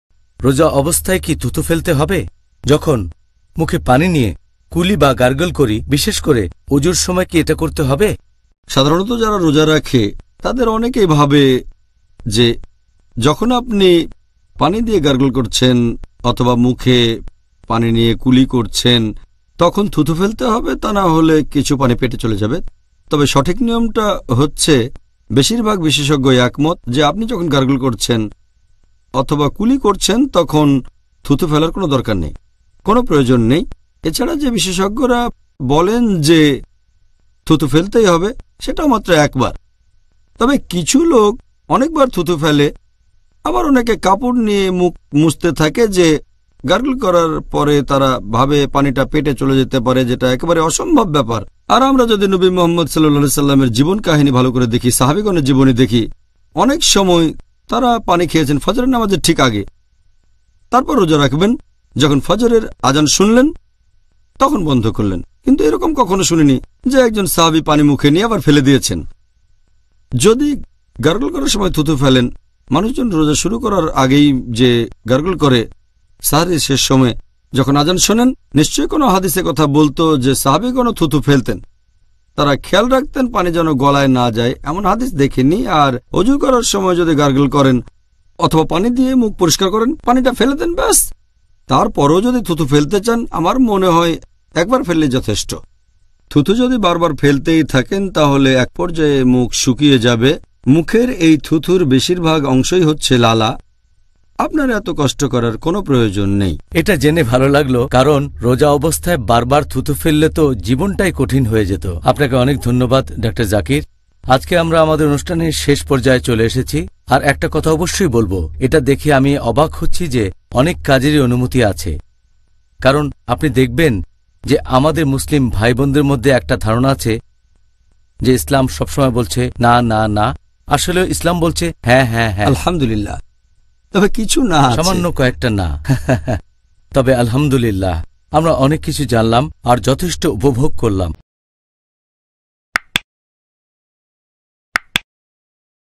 রোজা অবস্থায় কুলি করার পর বারবার থুথু ফেলা কি ঠিক?
Questions and answers from Zakir Naik

ভিডিও টি ভাল লাগলে অবশ্যই লাইক ও শেয়ার করুন যেন সবাই দেখার সুযোগ পায়।

কোন প্রশ্ন বা মতামত থাকলে কমেন্টে আপনার মূল্যবান মতামত দেওয়ার অনুরোধ রইল।